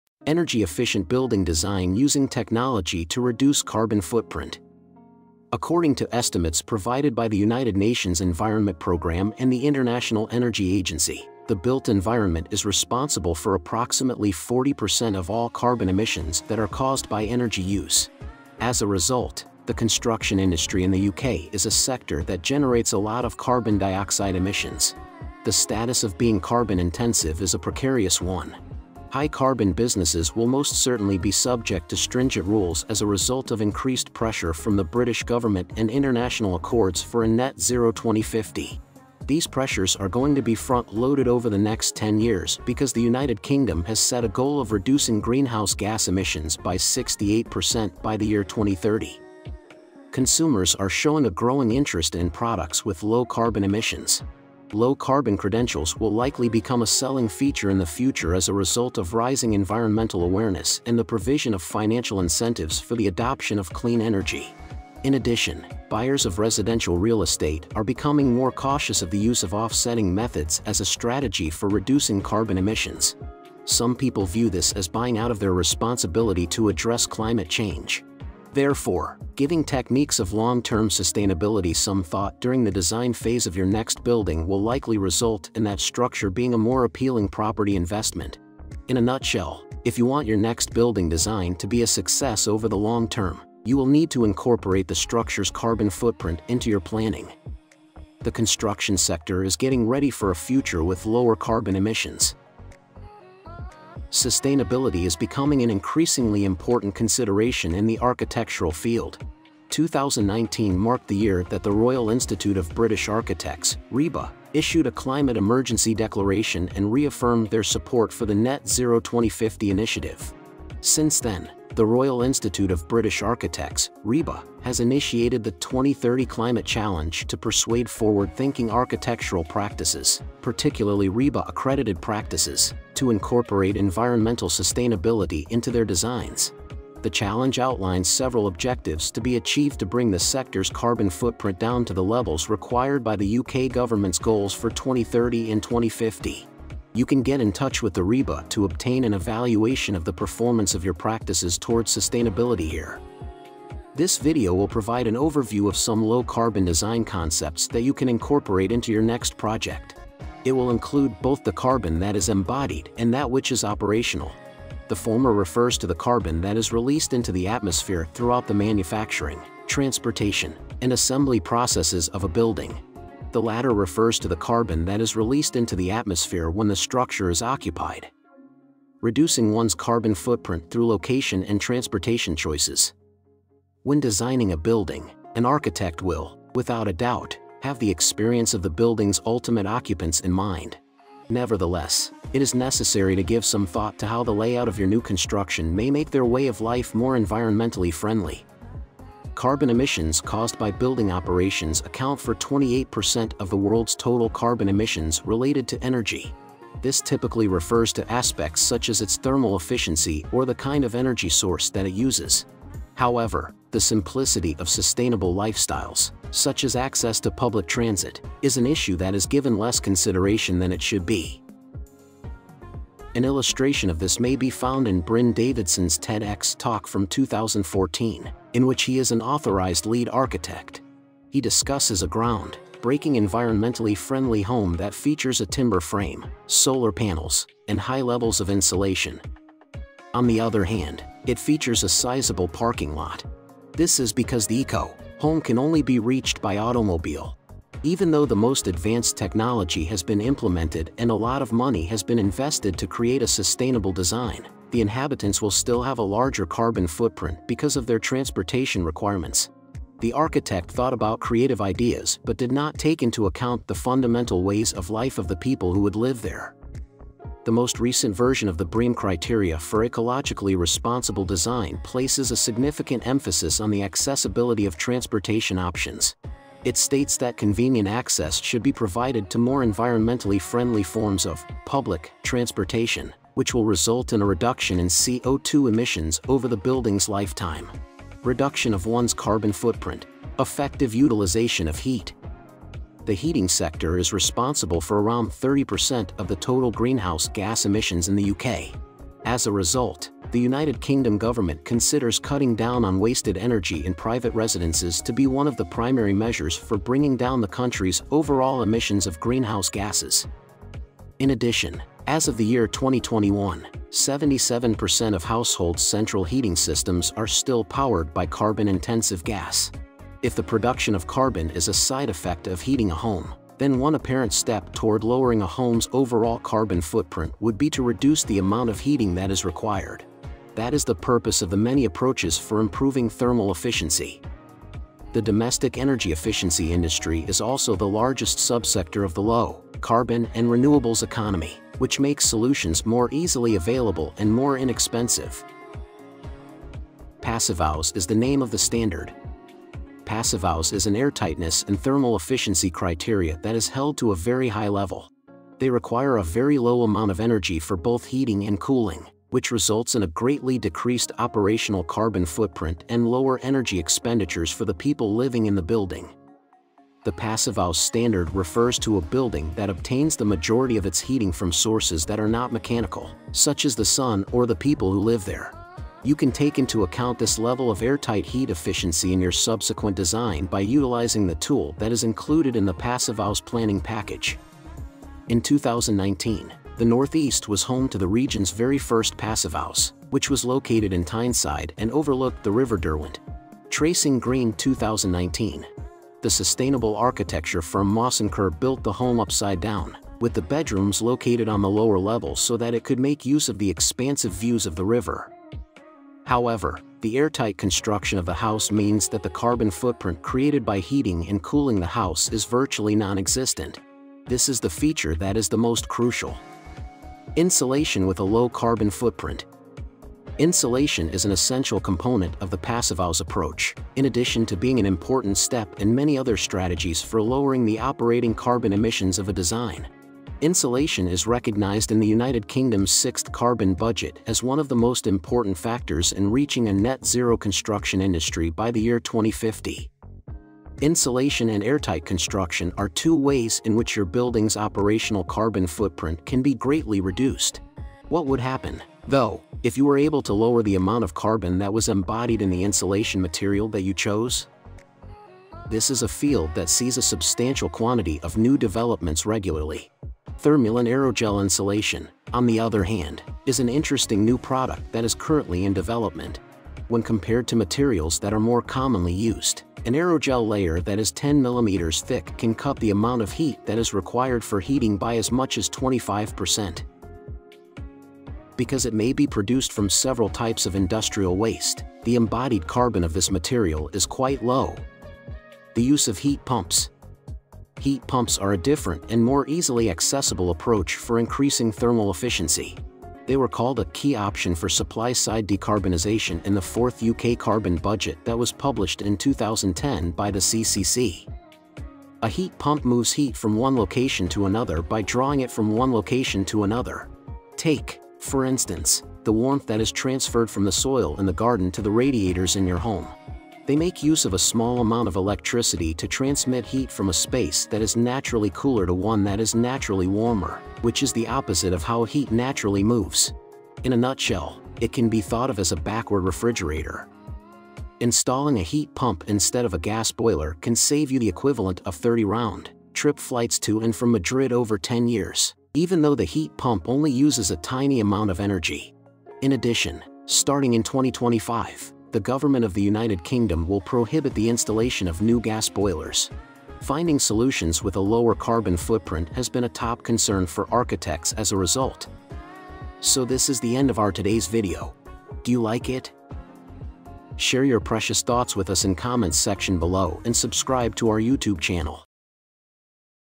Energy Efficient Building Design Using Technology to Reduce Carbon Footprint
In this video, we'll explore the latest technologies and design practices that are revolutionizing energy-efficient building design and helping to reduce our carbon footprint. From smart building systems to renewable energy sources, we'll showcase the incredible ways that technology is changing the way we think about sustainable building design.

We'll start by discussing the basics of energy-efficient building design, including the key factors that contribute to a building's carbon footprint. We'll then explore some of the innovative technologies that are being used to reduce energy consumption and improve building performance, including smart lighting systems, automated HVAC systems, and advanced insulation materials.

Next, we'll delve into some of the specific ways that renewable energy sources are being integrated into building design, including solar panels, wind turbines, and geothermal energy systems. We'll showcase real-world examples of buildings that are using these technologies to reduce their reliance on traditional energy sources and drastically reduce their carbon footprint.

Finally, we'll look to the future and discuss some of the exciting possibilities for energy-efficient building design, including the potential for buildings to generate more energy than they consume, and the use of advanced data analytics to optimize building performance and reduce energy waste.

Whether you're an architect, designer, or simply interested in the latest technologies and innovations in sustainable building design, this video is sure to inspire and inform. Join us as we explore the incredible ways that technology is helping to reduce our carbon footprint and create a more sustainable future.